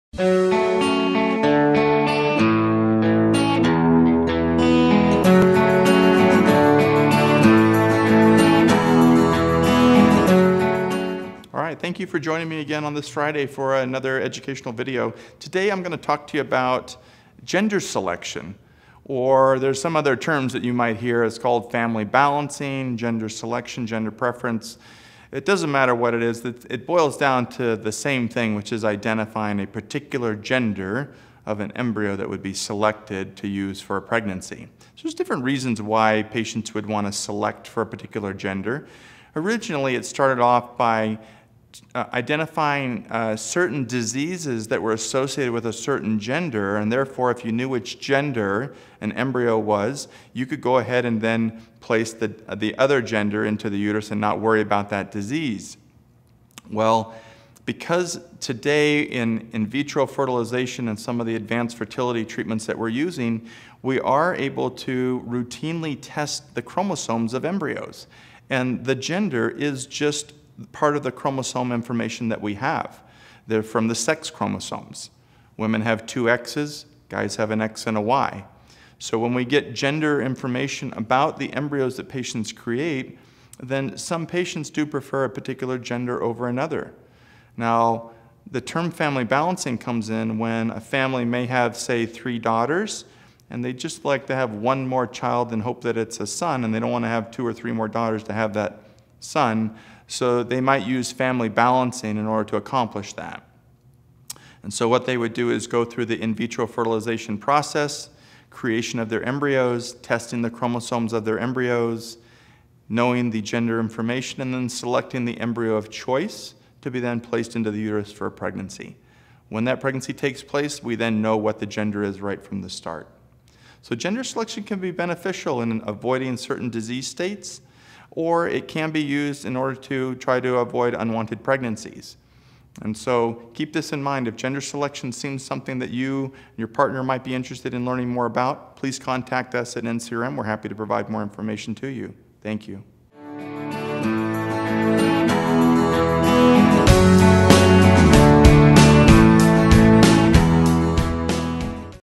Fertility: A Video Series S02 E10 - Gender Selection - Dr. Whitten
Another FertFriday video coming your way! This week Dr. Whitten explains what gender selection is and what this option could be used for. If you are curious about gender selection, be sure to check it out.